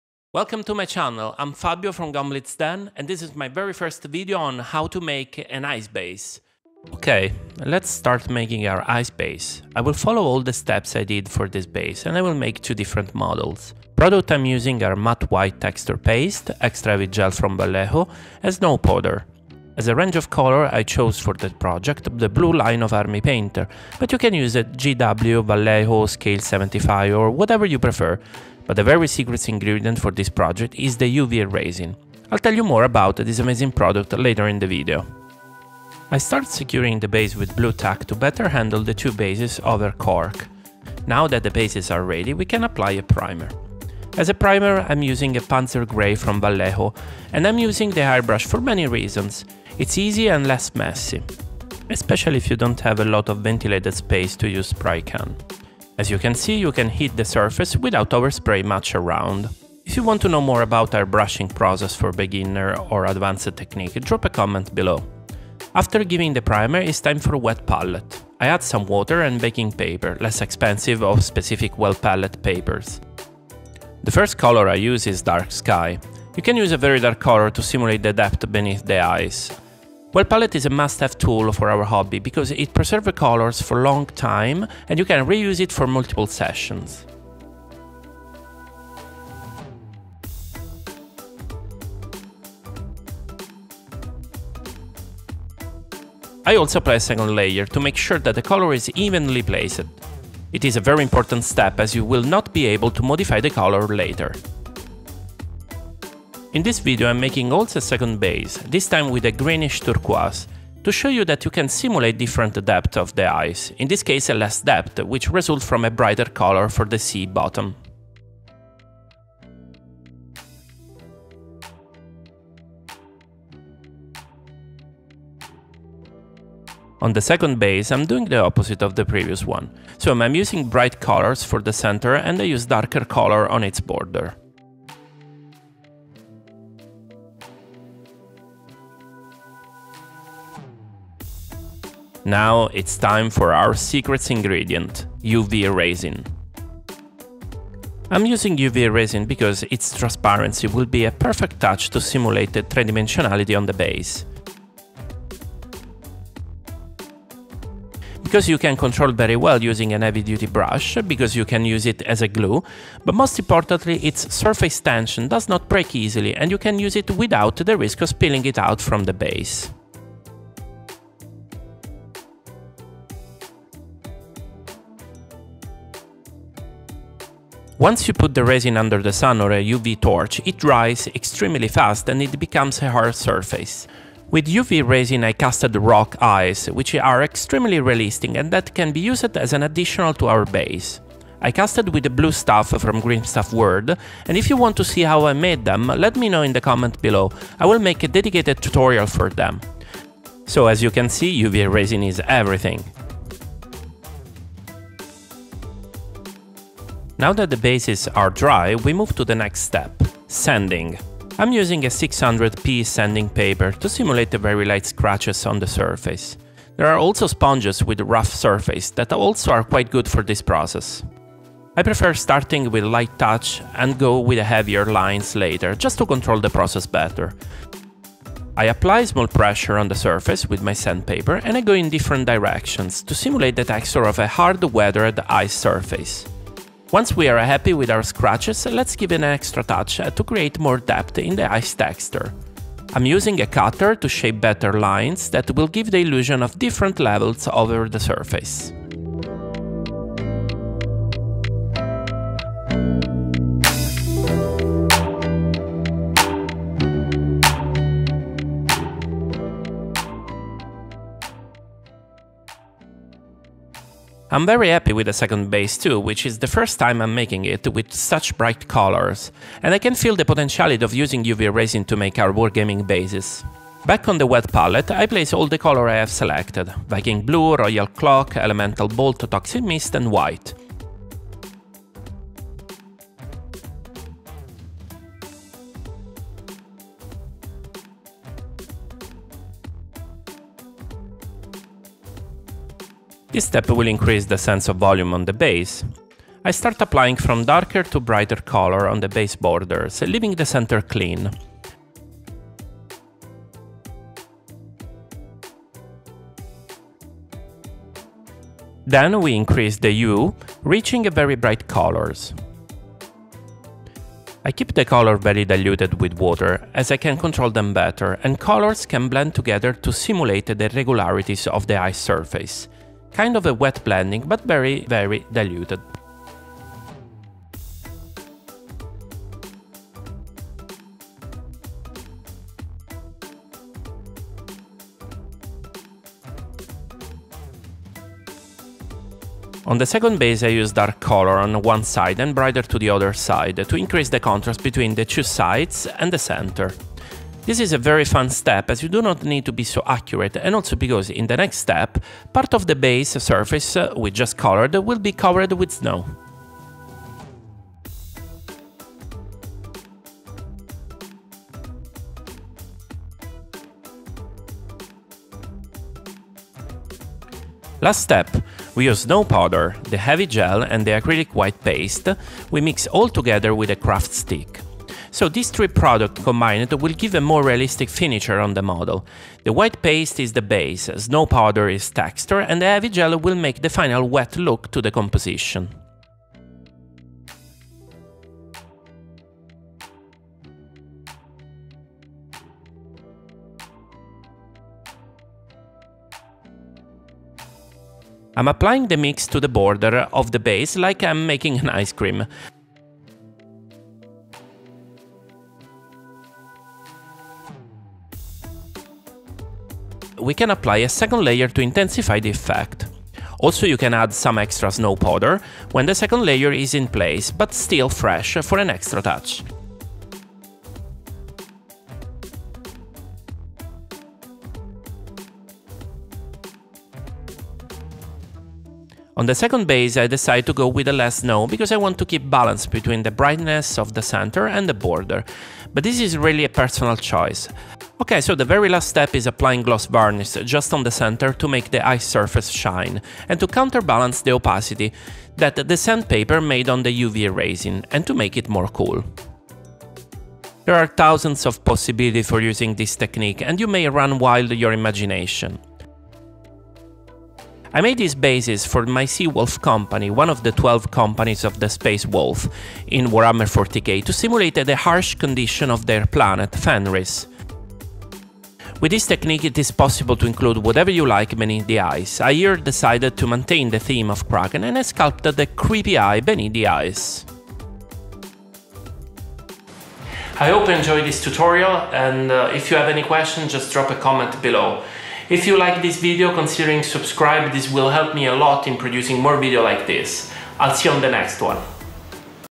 How to make the most REALISTIC ICE and SNOW EFFECT for miniature bases – STEP BY STEP
Ice is arguably one of the most challenging scenic base and terrain to replicate during our miniature painters’ journey. There are many products out there that claim to be “realistic,” but I always struggled to use them, at least until I discovered my recipe. In this tutorial, I am showing how to make super realistic ice snowed bases in less than 10 minutes.

🔮Products I used:
1️⃣ Extra Heavy Gel by Vallejo, however, any heavy gel does the job! Alternatively, you can use GOLDEN Heavy Gel Gloss

2️⃣ UV Resin and UV Lamp
3️⃣White acrylic paste: you can get it in any art store or on the internet

4️⃣Paint used:
Army painter
Toxic Mist
Viking Blue
Royal Cloak
Elemental Bolt
Matt White
Gloss Varnish (Green Stuff World)